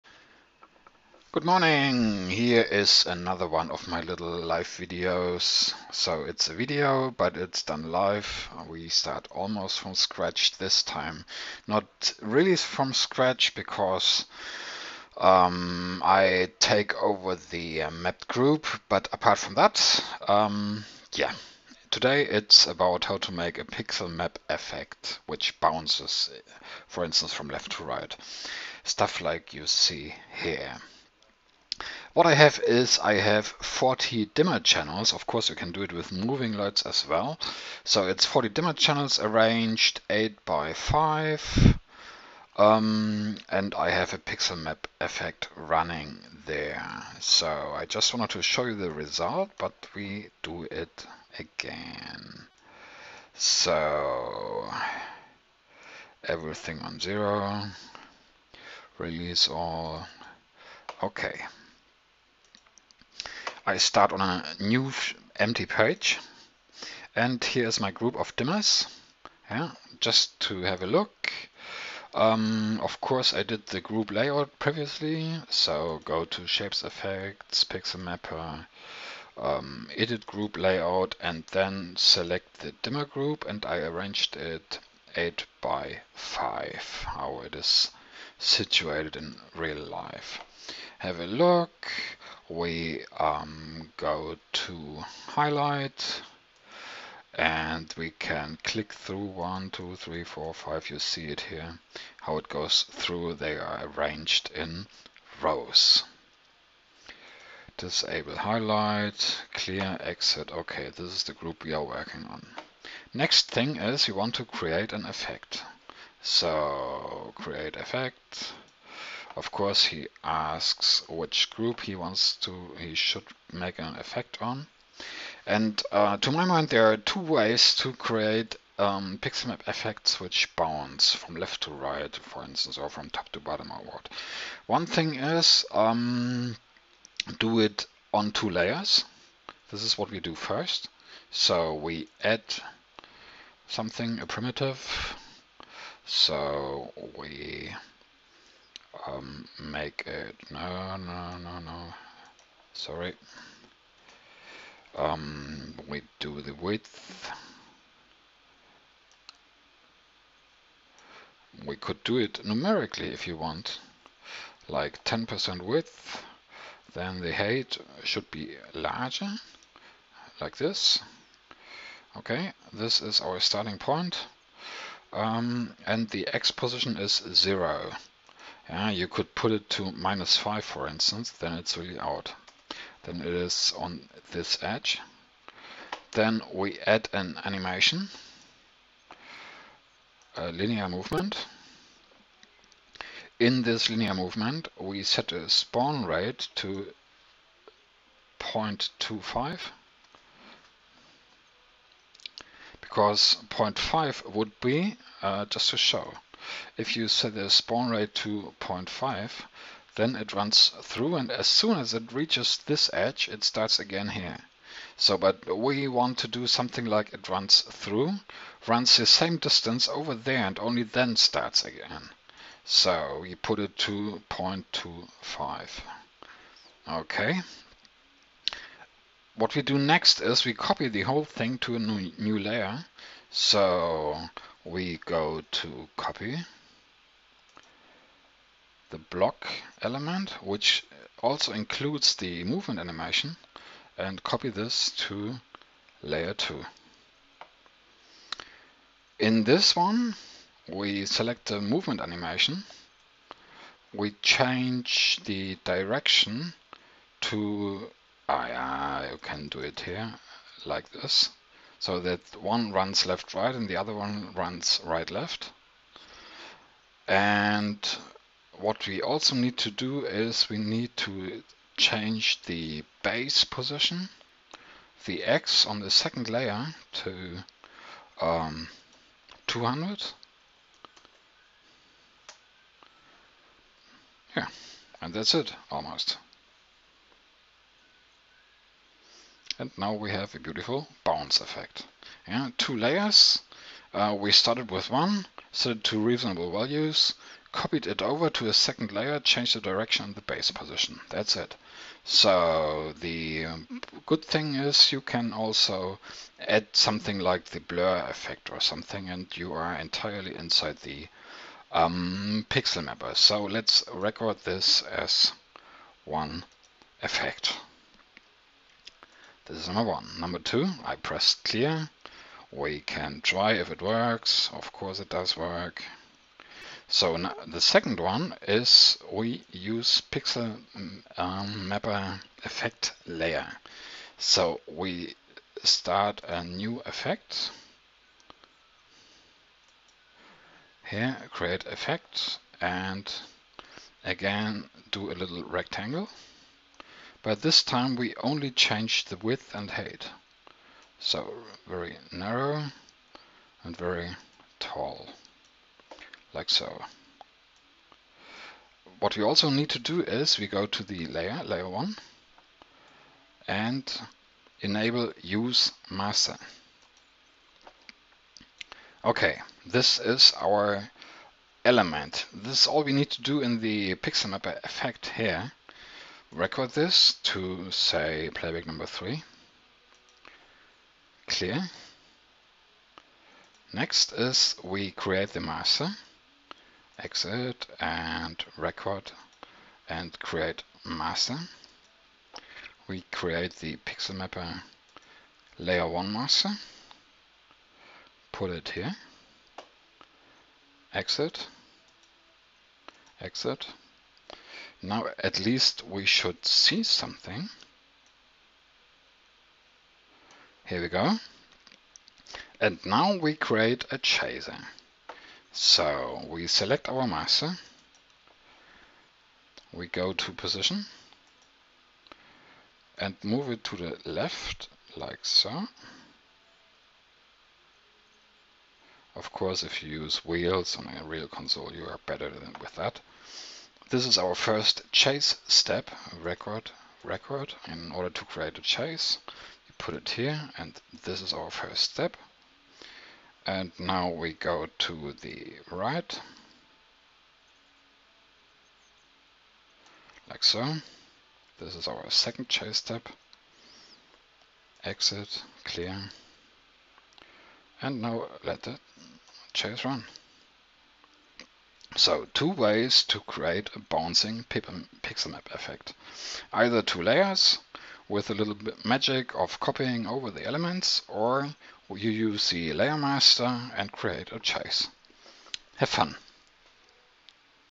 pixelmap bounce
How to create a bouncing pixelmap effect:
1) create and animate an effect, copy it to another layer, change direction and base position
OR
2) create an effect, enable Use Master, create this layer's master, and make a chase which bounces this